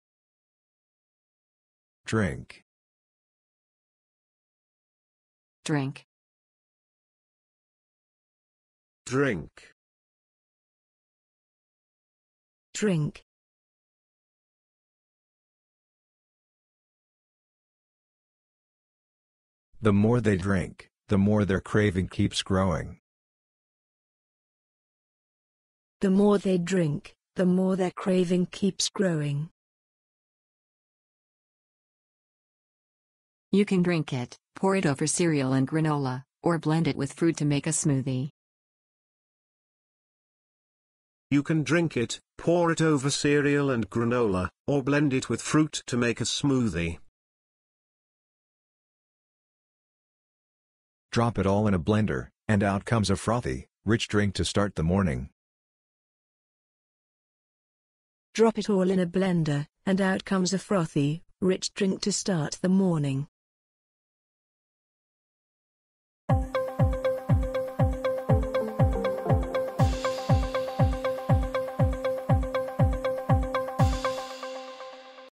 DRINK pronunciation | Improve your language with bab.la
Improve your spoken English by listening to DRINK pronounced by different speakers – and in example sentences too. Learn and love languages with bab.la.